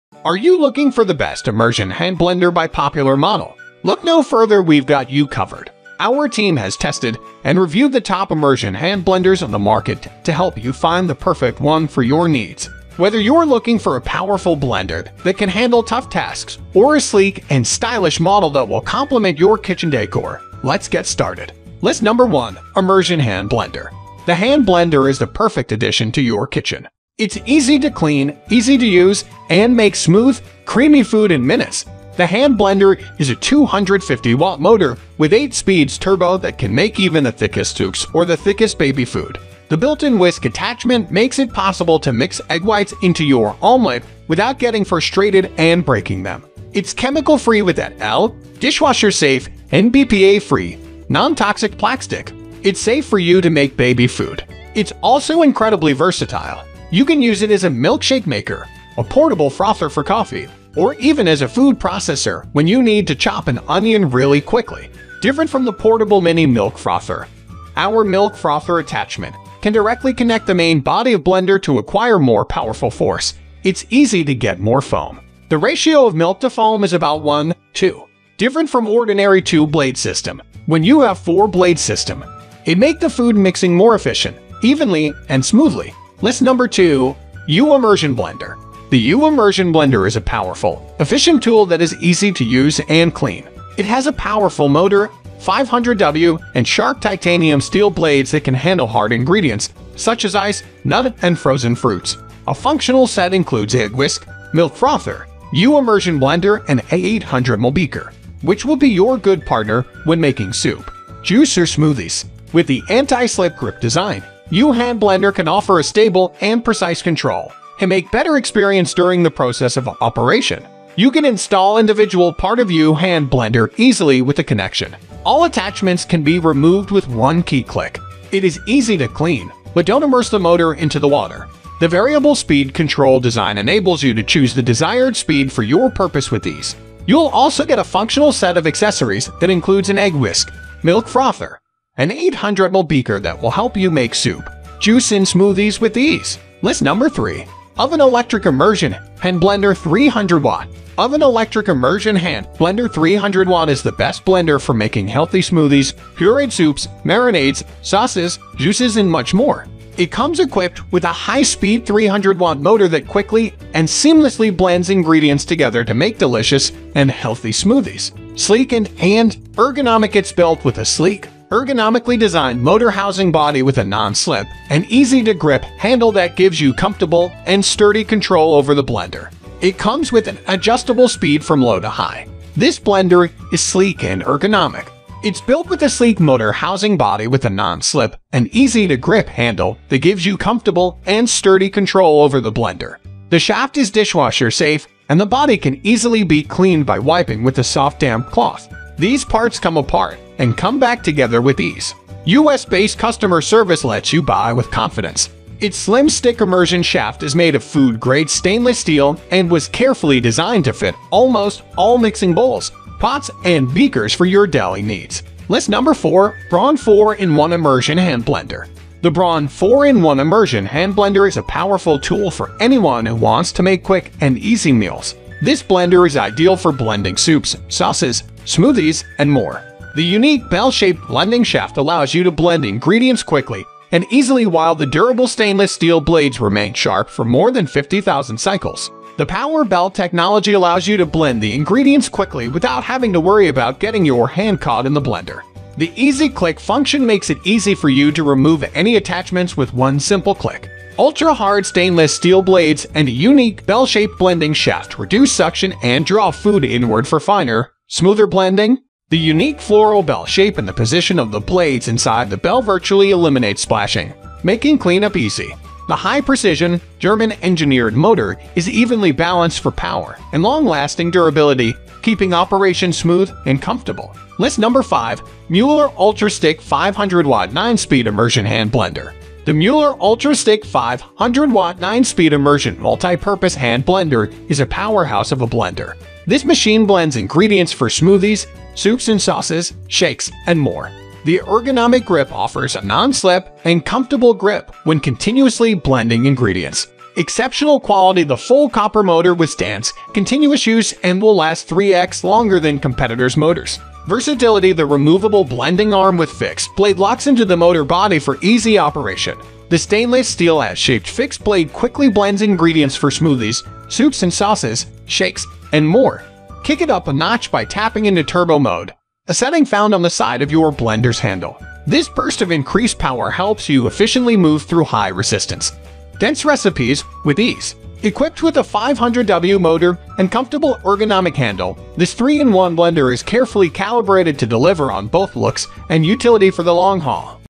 TOP 5: Best Immersion Hand Blender 2023 | Top Immersion Hand Blender Reviews
In this video, we have listed the top 5 Best Immersion Hand Blender on Amazon Reviews and buying Guide 2023

1. Immersion Hand Blender, UTALENT 3-in-1 8-Speed Stick Blender with Milk Frother, Egg Whisk for Coffee Milk Foam
2. YIOU Immersion Blender, Ultra-Stick Hand Blender Variable Speed Hand Blender 500 Watt Heavy Duty
3. Ovente Electric Immersion Hand Blender 300 Watt 2 Mixing Speed with Stainless Steel Blades
4. Braun 4-in-1 Immersion Hand Blender, Powerful 350W Stainless Steel Stick Blender, Multi-Speed
5. Mueller Ultra-Stick 500 Watt 9-Speed Immersion Multi-Purpose Hand Blender Heavy Duty